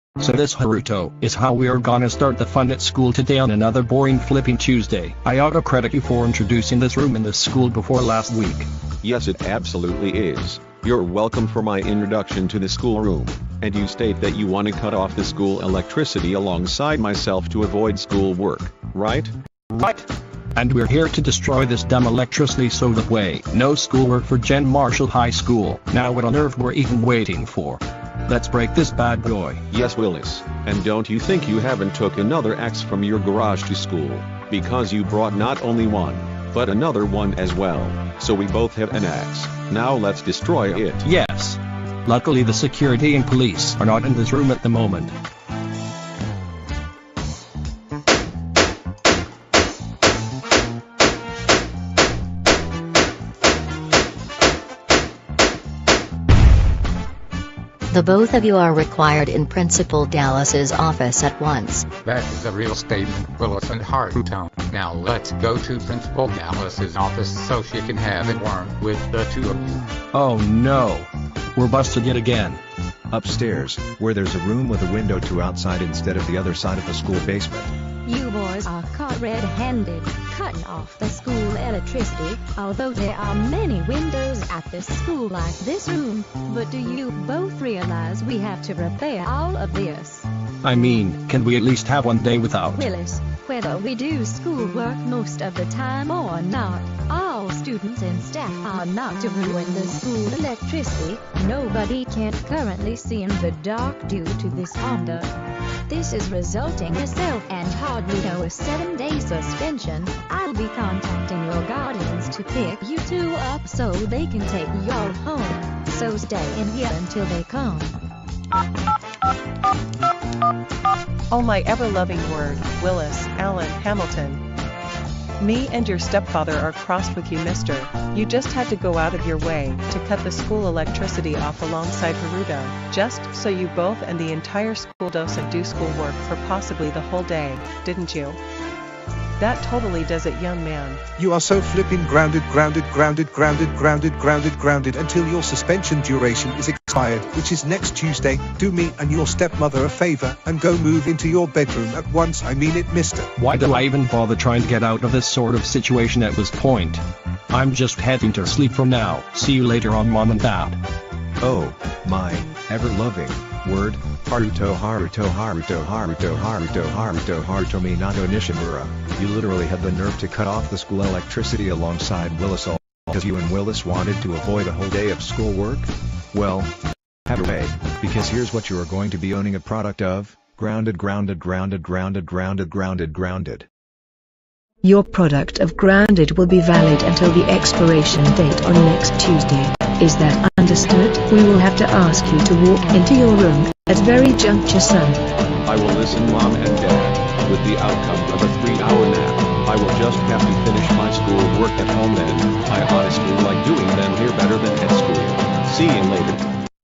Willis and Haruto cuts off the school electricity then gets grounded
Reminder:Haruto parents' phrase is inspired by @callumrockley854's grounding phrases because this video and his channel both mention "you are a owner of a product called grounded,and it will be valid until a certain day",but some of them was my own words.

The inspiration also goes to @ridenourpictures since Phillp Dawson repeated Dylan Dawson's name a lot when getting grounded just like Haruto's father repeated Haruto's in this video and the previous grounded Chris and Haruto sets up a Snow Day with magic/grounded.

I just wanted to give these creators credits because I inspire them,I oughta credit them since they are the original creators of the phraees,and hopefully I'm allowed to use them on my channel even more.